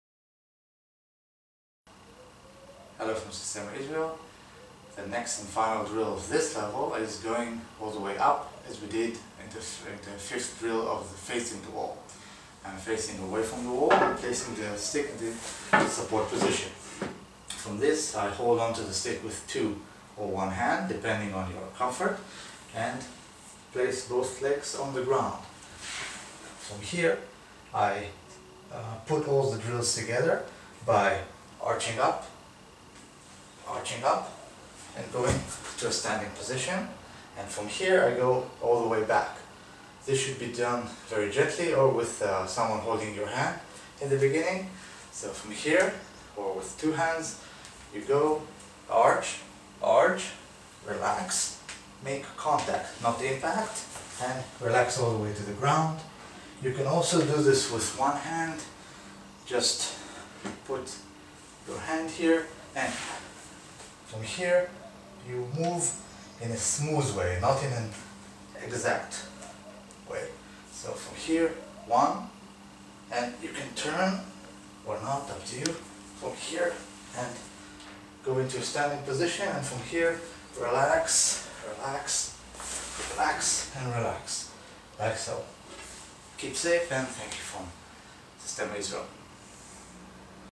Systema Israel Stick course 1 drill 10 of 10 Arching up and down with support And dynamic tension
In this video, we show how to easily and with no cost create movements and loading that promotes a full healthy body. Do the drill with care and enjoy good health movement and energy at any age.